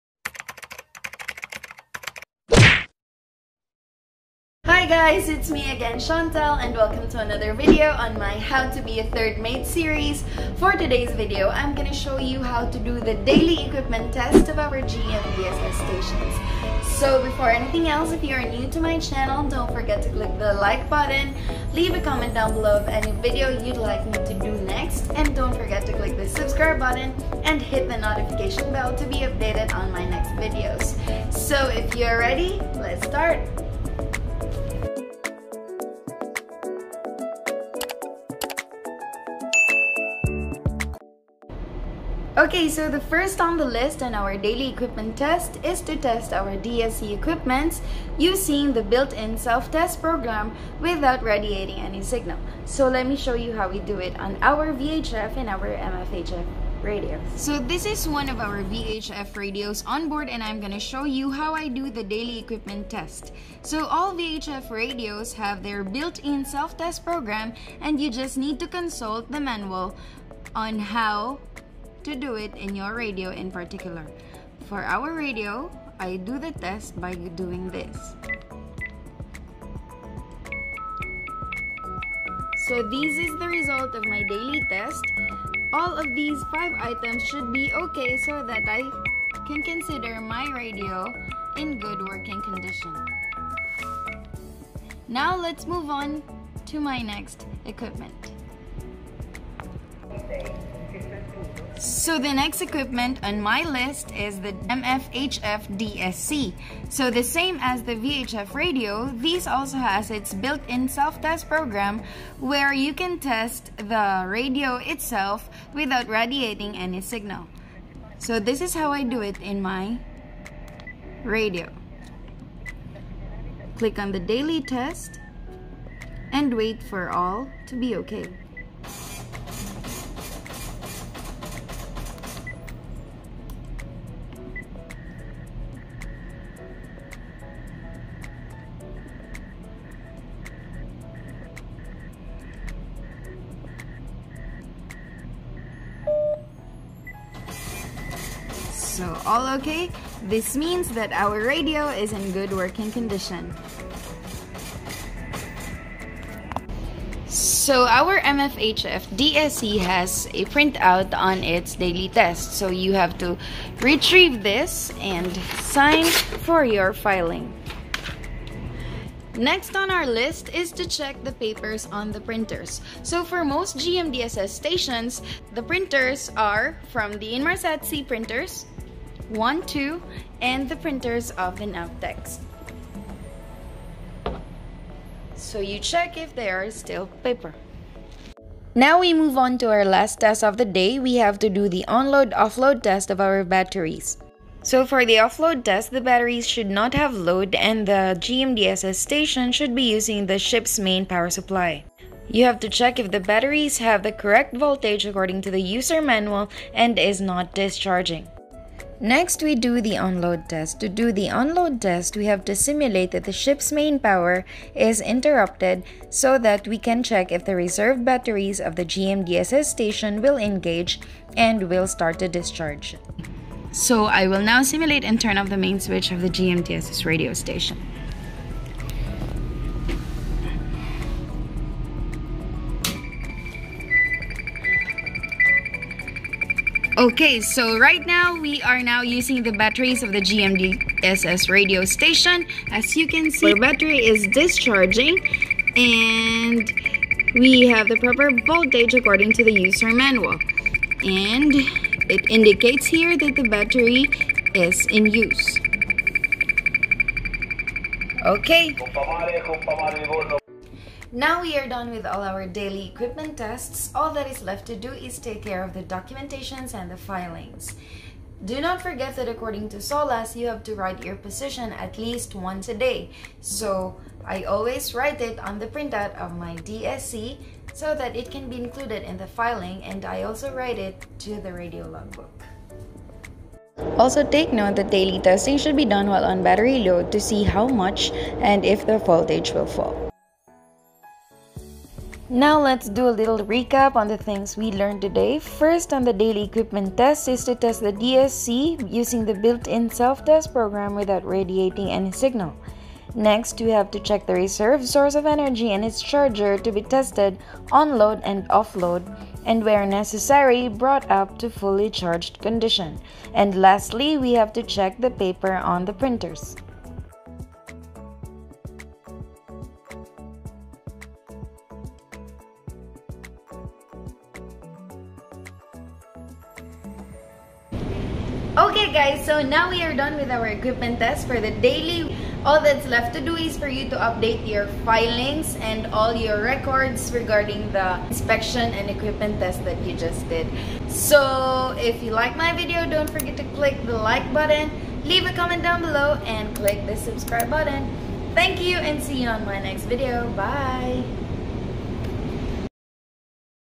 VLOG NO. 7 - HOW TO DO DAILY EQUIPMENT TEST OF THE GMDSS STATION
Hello 3rds 👋
I am very happy to be back with another video on my HowToBeAThirdMateSeries . This is a request video from one of the comments on my previous vlogs. Hope you like it. 👍

👉 INSTRUCTIONS FOR DAILY TEST

✅ DSC facilities to be tested by use of built in test program without radiating any signal.
✅ The reserve source of energy (radio batteries) and its charger to be tested on-load and off-load, and where necessary brought up to the fully charged conditions.
✅ Printers, check of paper.

Would like to say thanks to the people who helped me with this vlog.
Manager: Macoy
Quality Check: Capt Truels
Adviser: Sir Sveinung